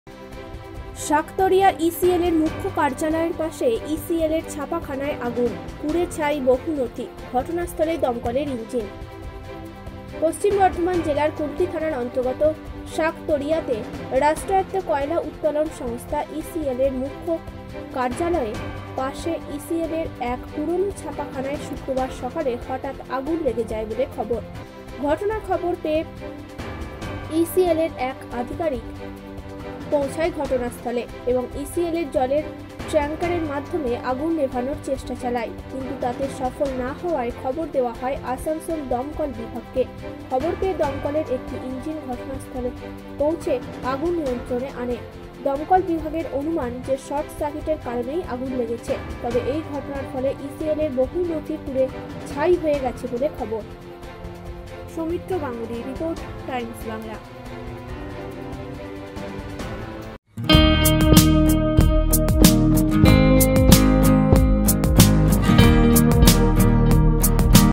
সাঁকতড়িয়া ইসিএলের মূখ্য কার্যালয়ের পাশে ইসিএলের ছাপাখানায় আগুন , পুড়ে ছাই বহু নথি
সাঁকতড়িয়া ইসিএলের মূখ্য কার্যালয়ের পাশে ইসিএলের ছাপাখানায় আগুন , পুড়ে ছাই বহু নথি , ঘটনাস্থলে দমকলের ইঞ্জিন